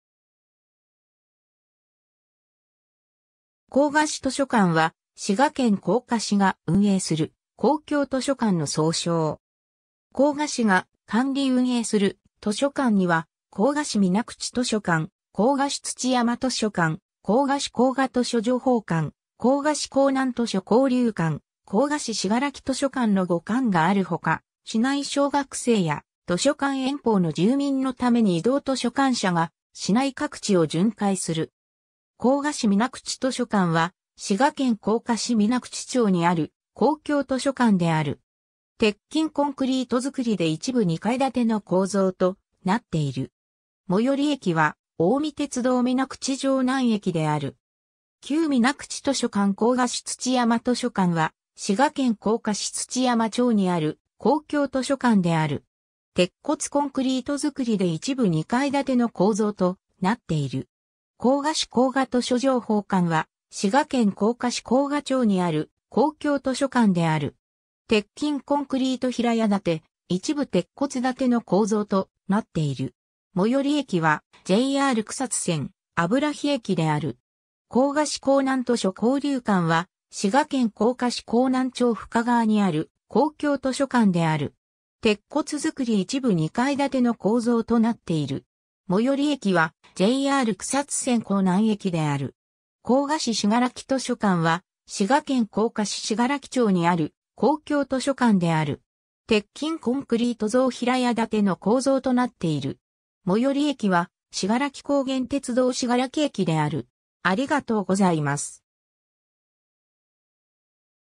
甲賀市図書館
甲賀市図書館（こうかしとしょかん）は、滋賀県甲賀市が運営する公共図書館の総称。
 甲賀市が管理運営する図書館には、甲賀市水口図書館、甲賀市土山図書館、甲賀市甲賀図書情報館、甲賀市甲南図書交流館、甲賀市信楽図書館の5館がある他、市内小学生や図書館遠方の住民のために移動図書館車が市内各地を巡回する。
 甲賀市水口図書館（こうかしみなくちとしょかん）は、滋賀県甲賀市水口町にある公共図書館である。
 鉄筋コンクリート造で一部2階建ての構造となっている。
最寄り駅は近江鉄道水口城南駅である。
 旧水口図書館 甲賀市土山図書館（こうかしつちやまとしょかん）は、滋賀県甲賀市土山町にある公共図書館である。
 鉄骨コンクリート造で一部2階建て（中2階）の構造となっている。
 甲賀市甲賀図書情報館（こうかしこうかとしょじょうほうかん）は、滋賀県甲賀市甲賀町にある公共図書館である。
 鉄筋コンクリート平屋建、一部鉄骨建の構造となっている。
最寄り駅はJR草津線油日駅である。
 甲賀市甲南図書交流館（こうかしこうなんとしょこうりゅうかん）は、滋賀県甲賀市甲南町深川にある公共図書館である。
 鉄骨造一部2階建の構造となっている。
最寄り駅はJR草津線甲南駅である。
 甲賀市信楽図書館（こうかししがらきとしょかん）は、滋賀県甲賀市信楽町にある公共図書館である。
 鉄筋コンクリート増平屋建（一部地下）の構造となっている。
最寄り駅は信楽高原鐵道信楽駅である。